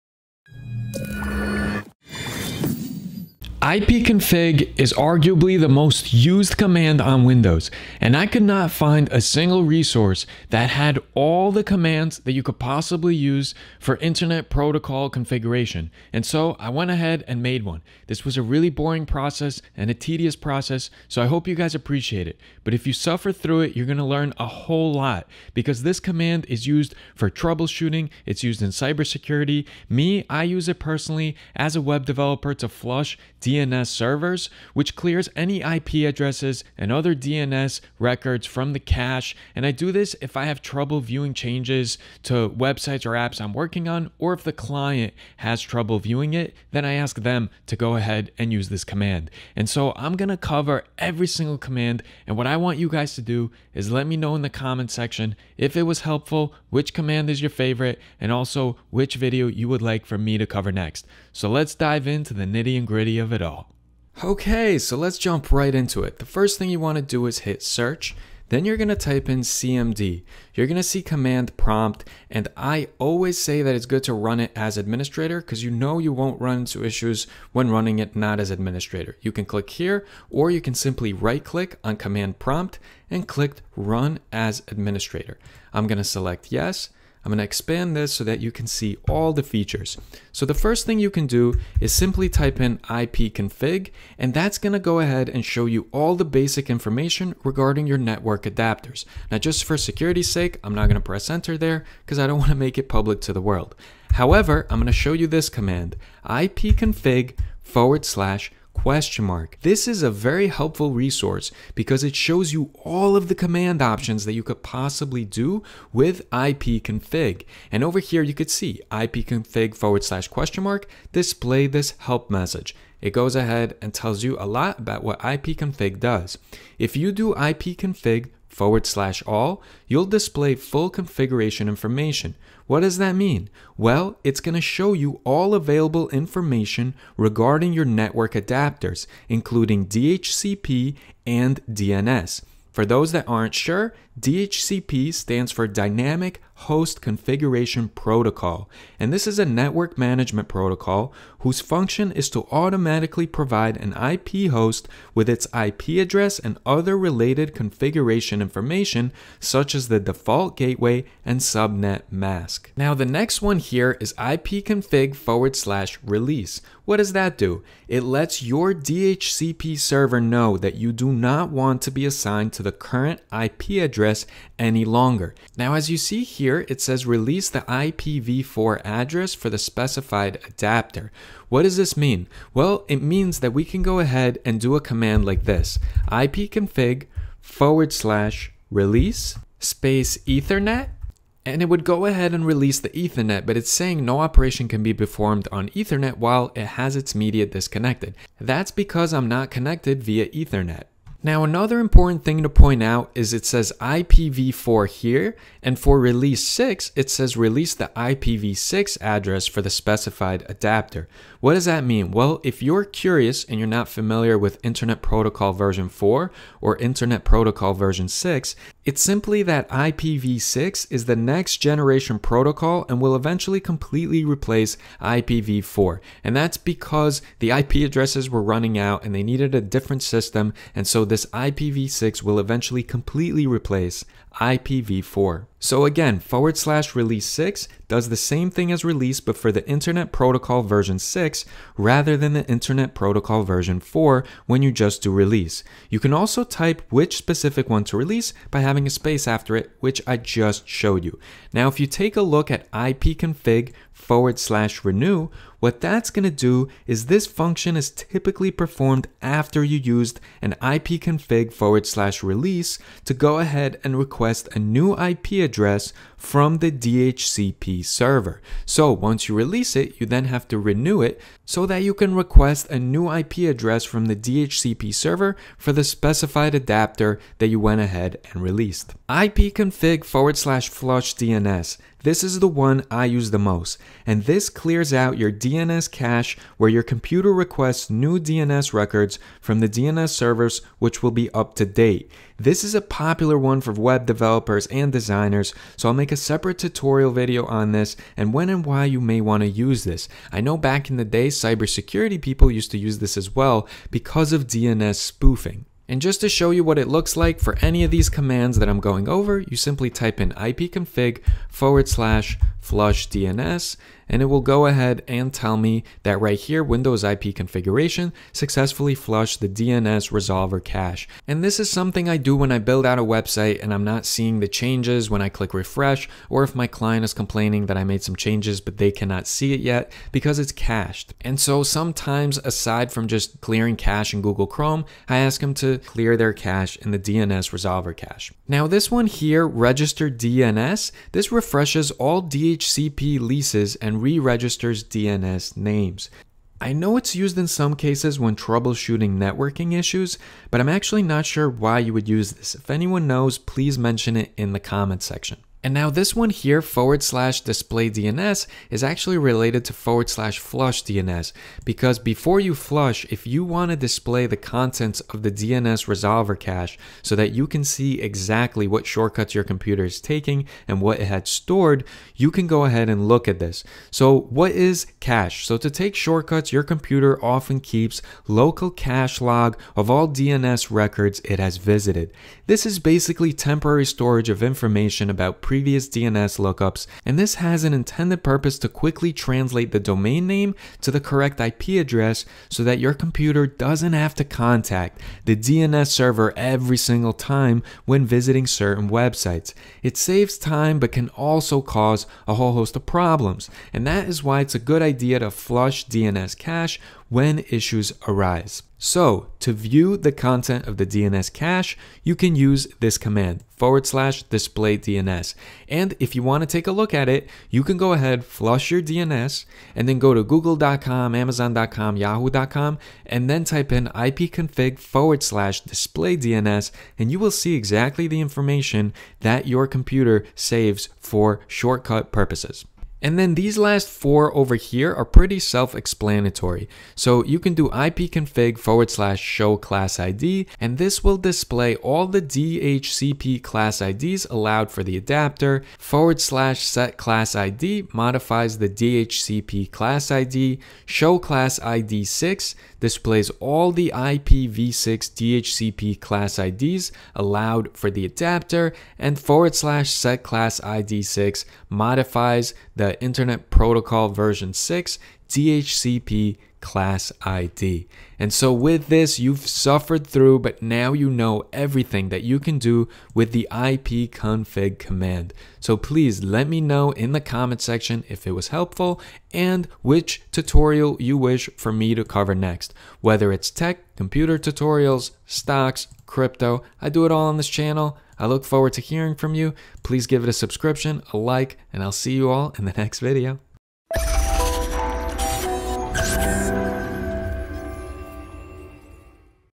IPConfig Command Explained in Full Tutorial
ipconfig is likely the most used command for troubleshooting networking issues. It's used a lot by web developers, designers, networking admins and cybersecurity specialists as well.

Ipconfig shows basic network information regarding your network adapters.
Ipconfig/all shows all available information regarding your network adapters including DHCP and DNS.
Ipconfig/release lets your DHCP server know that you do not want to be assigned to the current IP address any longer.
DHCP Dynamic Host Configuration Protocol (DHCP) this is a network management protocol whose function is to automatically provide an IP host with its IP address and other related configuration information such as the default gateway and subnet mask.
If you’re curious, IPV6 is the next generation protocol and will eventually completely replace IPV4
Ipconfig/release does the same thing as release but for the Internet Protocol Version 6 rather than the Internet Protocol Version 4 when you just do release.
You can also type which specific one to release by having a space after ipconfig/release wireless* or ipconfig/release ethernet
Ipconfig/renew is a function is typically performed after you used an ipconfig/release to go ahead and request a new IP address from the DHCP Server.
ipconfig/flushdns clears out your DNS cache where your computer requests new DNS records from the DNS servers which will be up to date. This is a popular one for web developers and designers so I’ll make a separate tutorial video on this and when and why you may you want to use this.
Ipconfig/registerdns I know it’s used in some cases when troubleshooting networking issue.
Ipconfig/display DNS To take a shortcuts, your computer often keeps local cache log of all DNS records it has visited. This temporary storage of information about previous DNS lookups has an intended purpose to quickly translate the domain names to the correct IP Address so that your computer doesn’t have to contact the DNS server every single time when visiting certain websites. It saves time but can cause problems and that is why it’s a good idea to flush DNS cache when issues arise. To view the content of the DNS cache you can use the command ipconfig/displaydns

0:00 Intro
1:05 CMD Command Prompt
1:32 IPCONFIG
2:05 IPConfig ALL
2:25 DHCP
2:42 IPConfig Release
3:33 IPv3 vs IPv4
4:12 IPConfig Renew
4:39 IPConfig FlushDNS
6:15 Cache
6:55 IPConfig DisplayDNS
7:45 Closing Thoughts